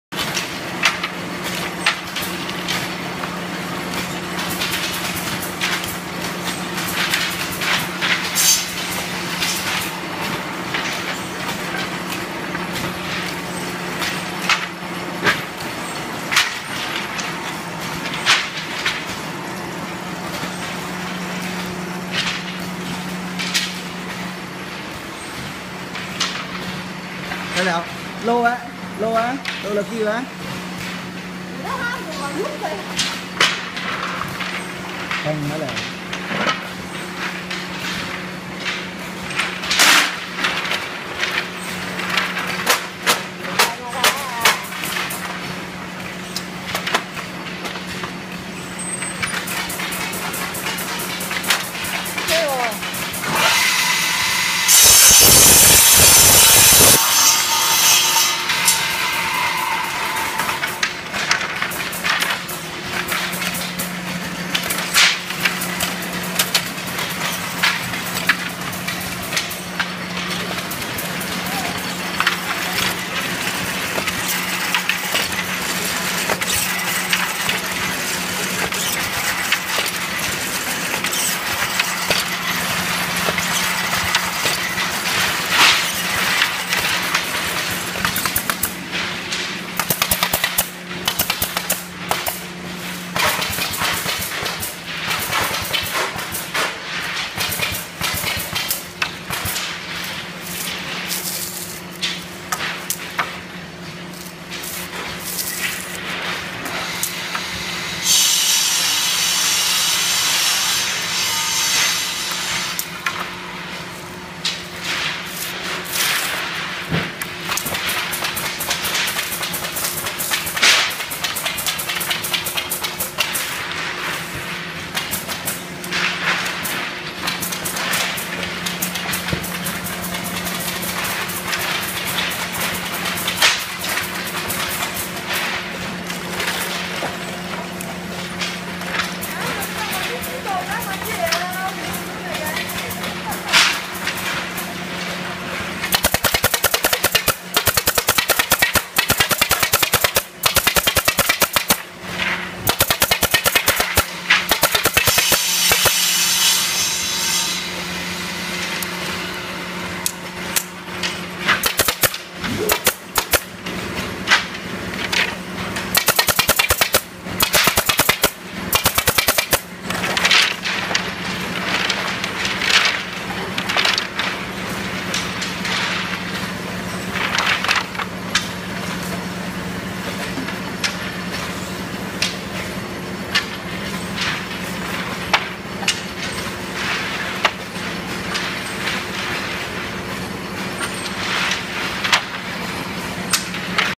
Wooden stick process from oem factory
Each wooden stick is under each process to provide you with good quality. Large production for quick delivery.